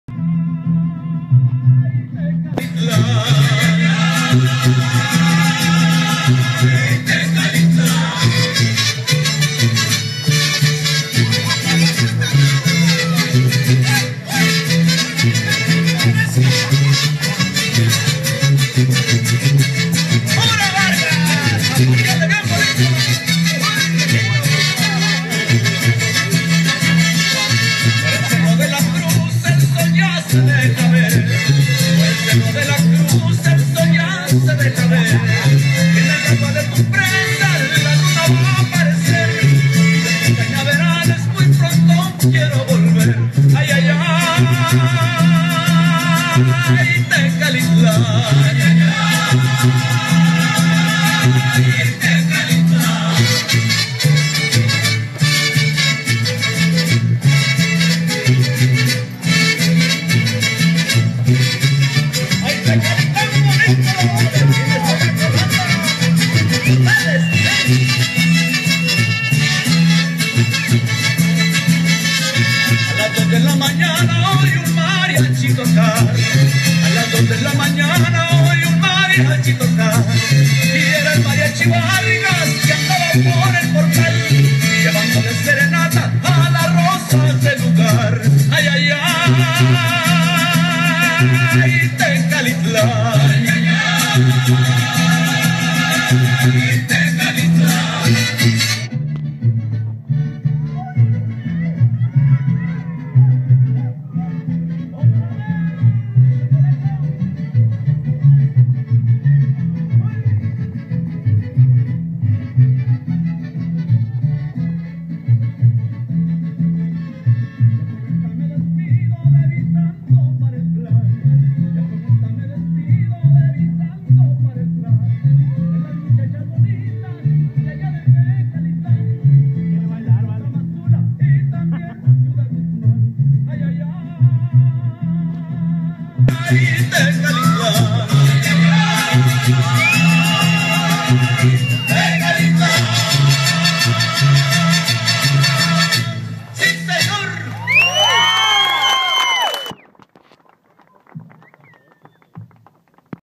Los Mestizos de San Jose Tecalitlan
Vero's 1st time dancing with the advanced group at age 6...so proud of her and the other kids.